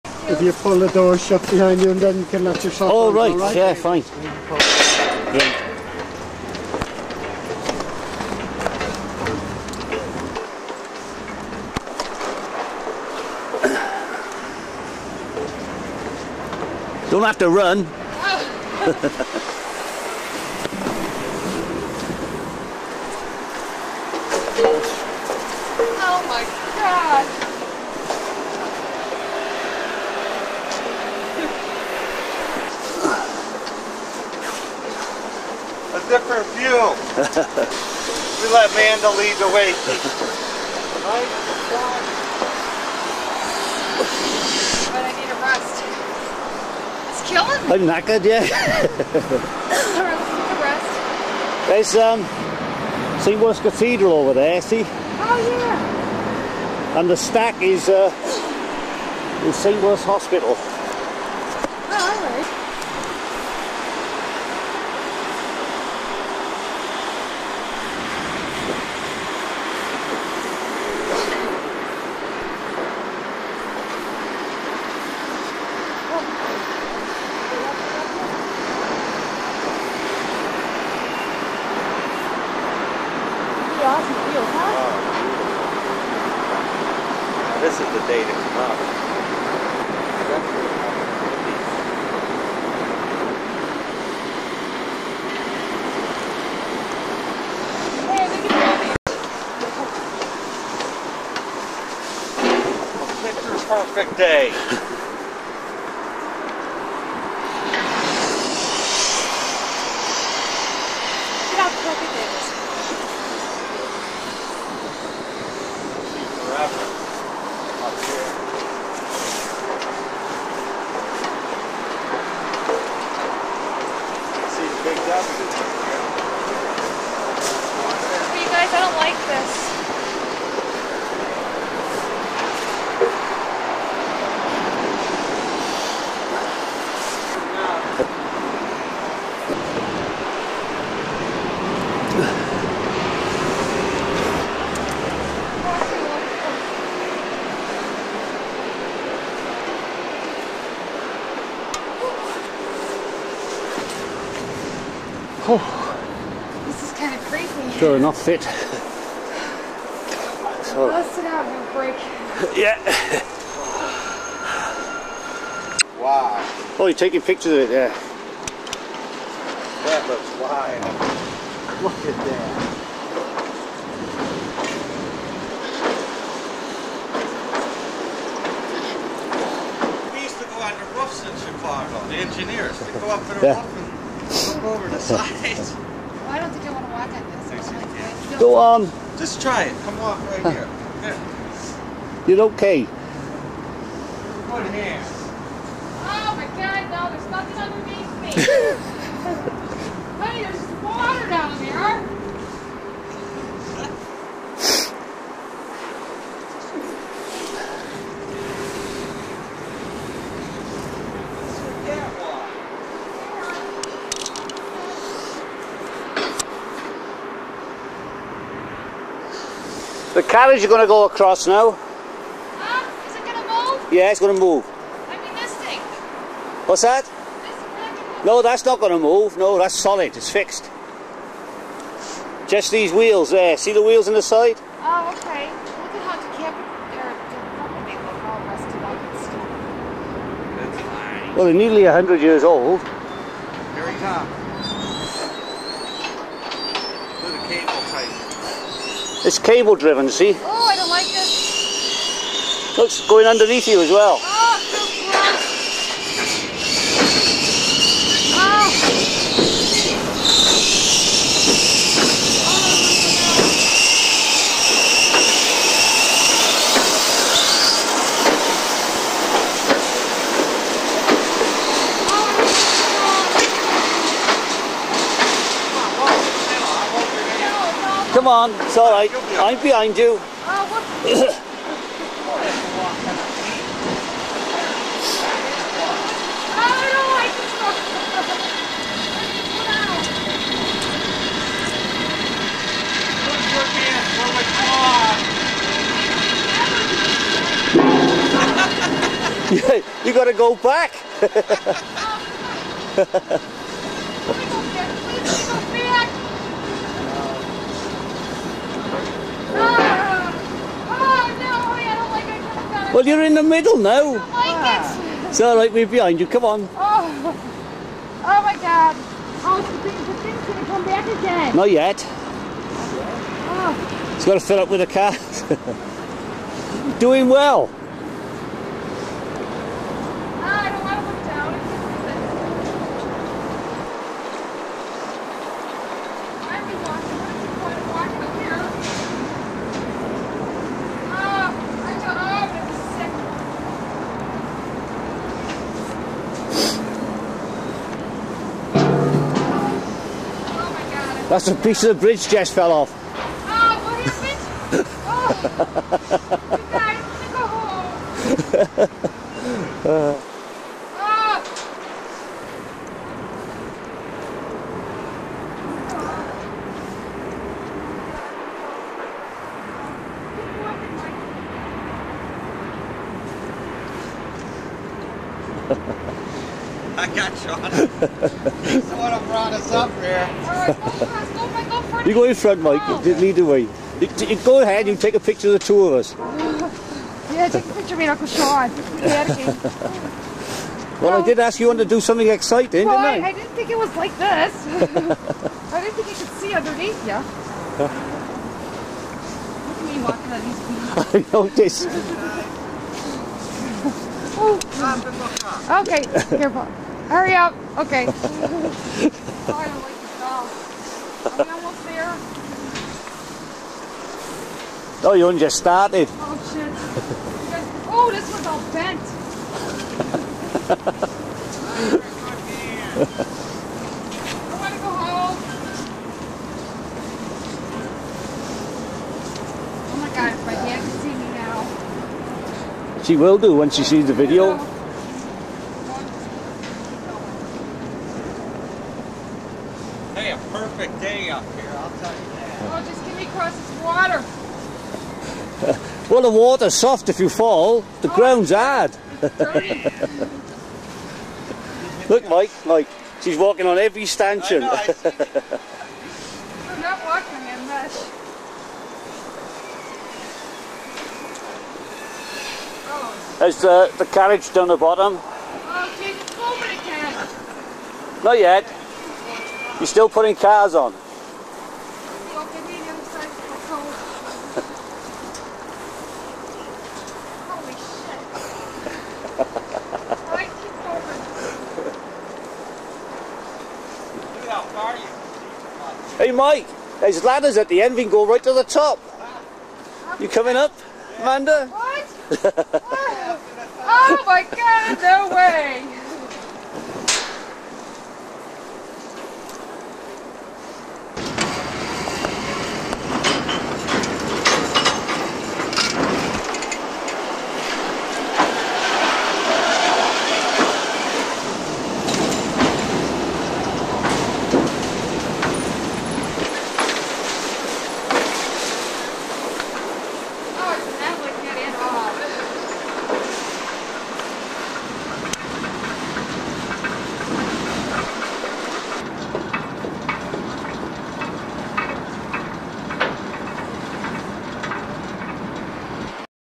Walk across the Transporter Bridge (Part 1)
A beautiful January day in 2003 and what better than a stroll across the top of Newport's famous Transporter bridge.